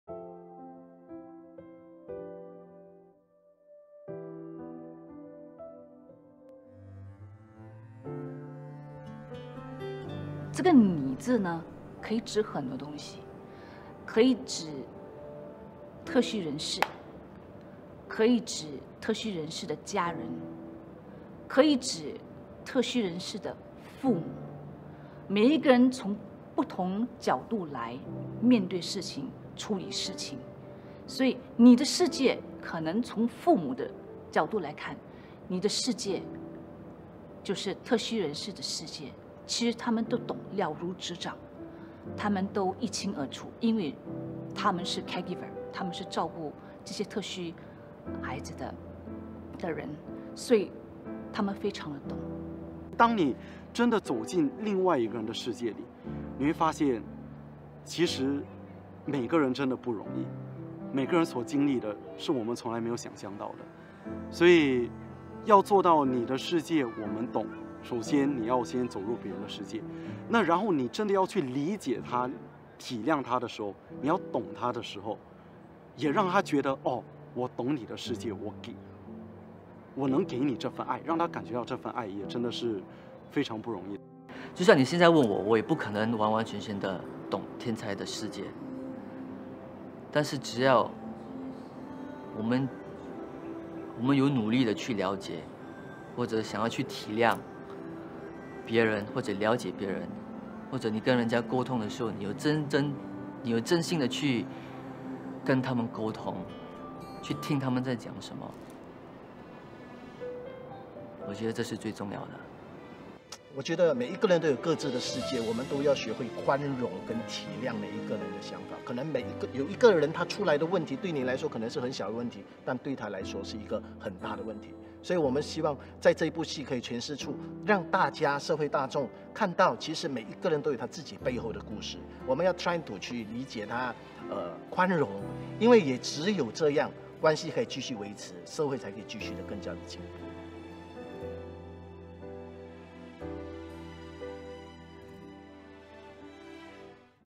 《你的世界我们懂》对艺人们有什么意义？ Interview with casts of 'Your World in Mine' 5 《你的世界我们懂》艺人访问 5
《你的世界我们懂》对艺人们有什么意义？
Does 'Your World in Mine' hold any special meaning to the casts?